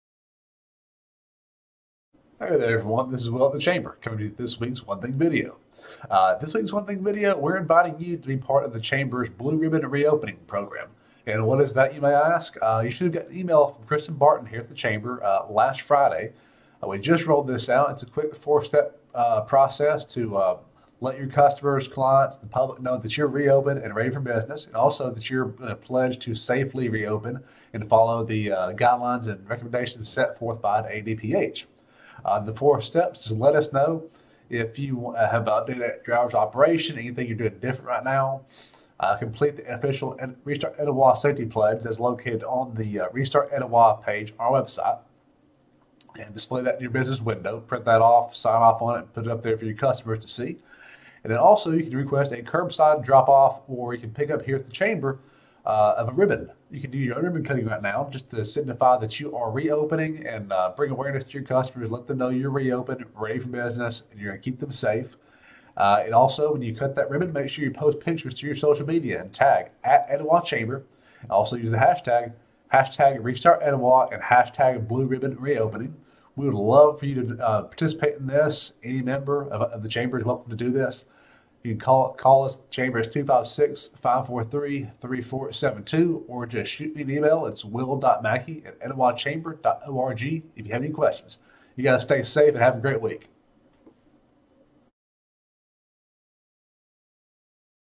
OneThing: ReStart Etowah Safety Pledge & Blue Ribbon Reopening 20200518
Join The Chamber for our Blue Ribbon Reopening Program and take the ReStart Etowah Safety Pledge!